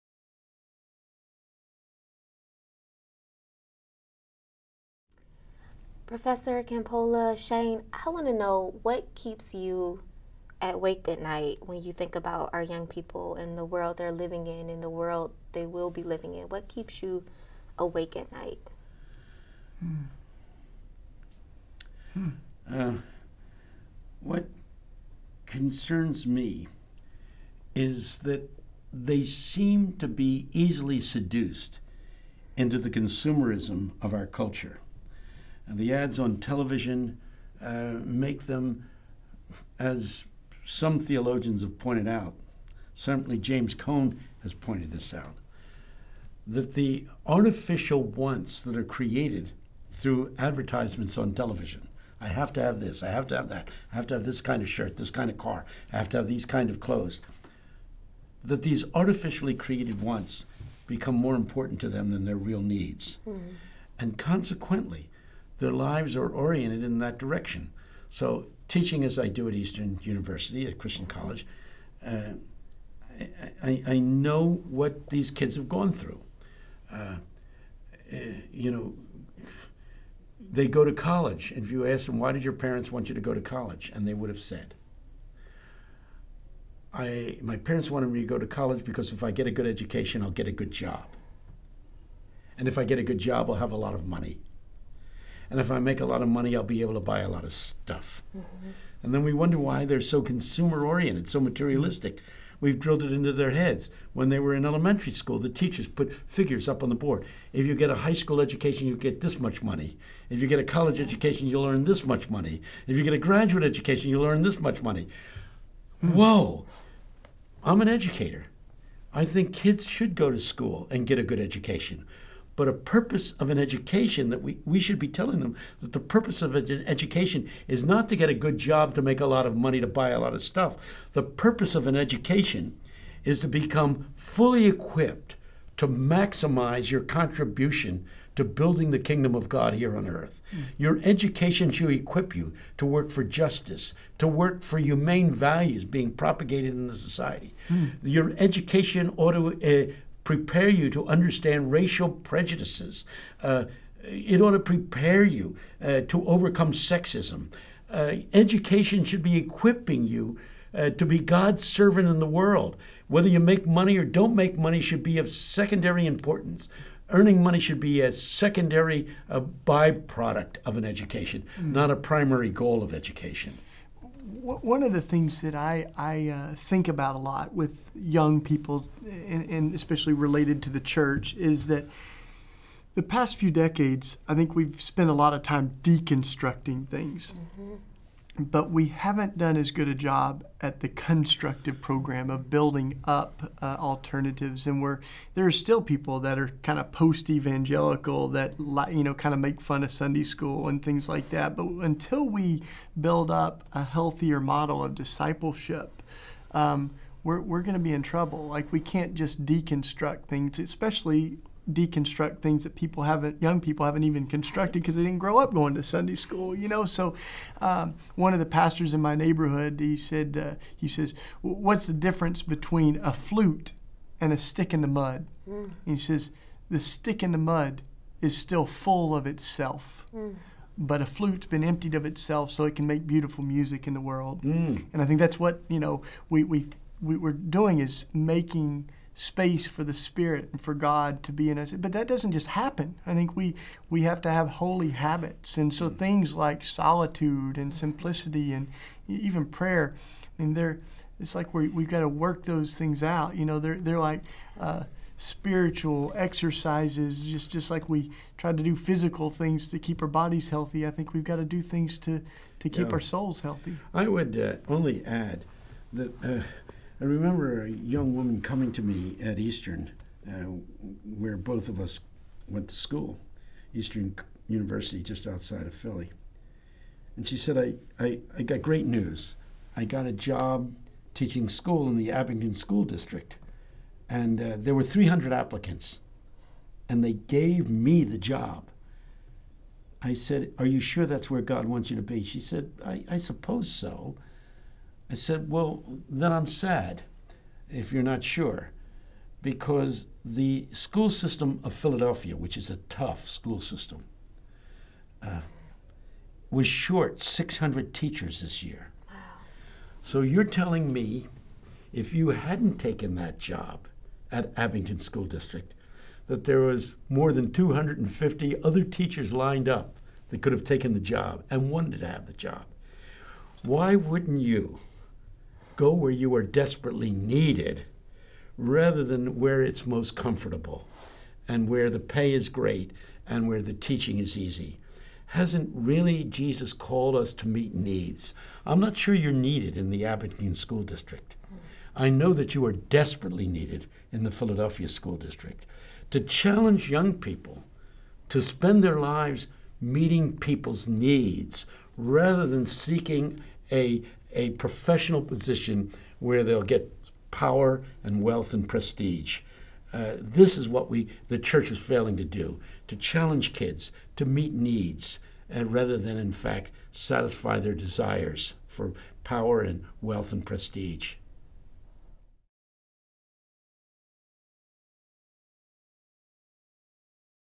Dr. Tony Campolo and Shane Claiborne - What Keeps You Up at Night?
Mr. Shane Claiborne and Dr. Tony Campolo share what keeps them up at night after delivering a lecture, “Abounding Joy: Grace and Its Consequences,” on Wednesday, April 4, 2018.

Dr. Tony Campolo is professor emeritus of sociology at Eastern University and a former faculty member at the University of Pennsylvania.  For 40 years, he lead the Evangelical Association for the Promotion of Education. More recently, Dr. Campolo has provided leadership for Red Letter Christians as well as for the Campolo Center for Ministry. He has written more than 35 books and can be found blogging regularly on tonycampolo.org and redletterchristians.org.

Mr. Claiborne writes and speaks around the world about peacemaking, social justice, and Jesus, and is the author of numerous books including Executing Grace. He is the visionary founder of The Simple Way in Philadelphia and director of Red Letter Christians.